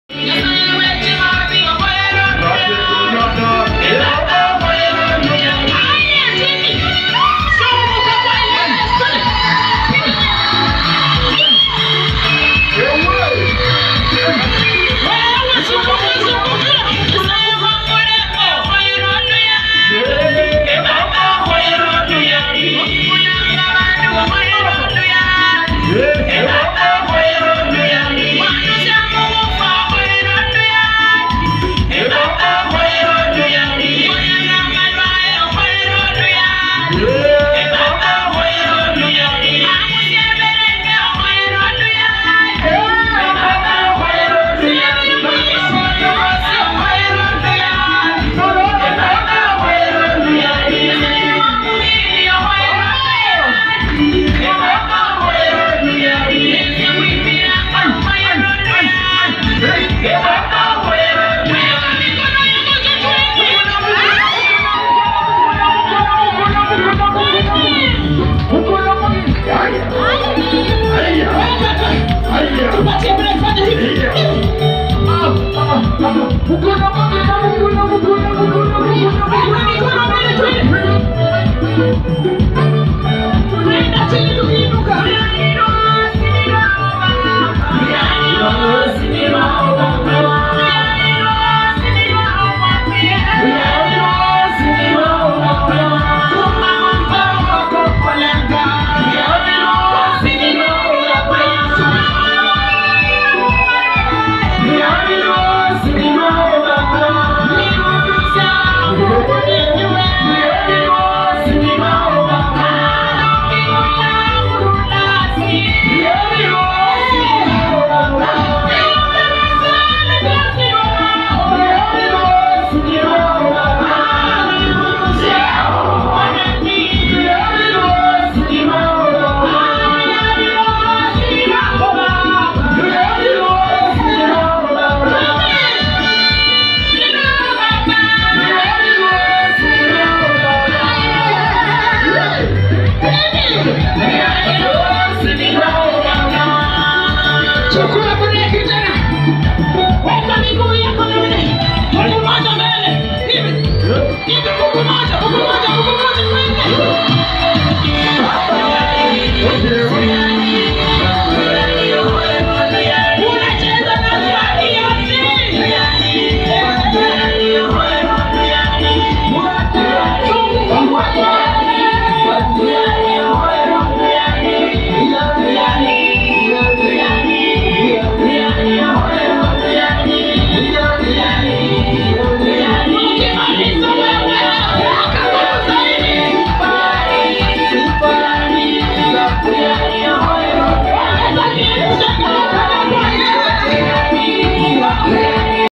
Performing live at The True Living Grace Ministries.
The song means Lord you are worthy of praise for everything that you continue doing in my life.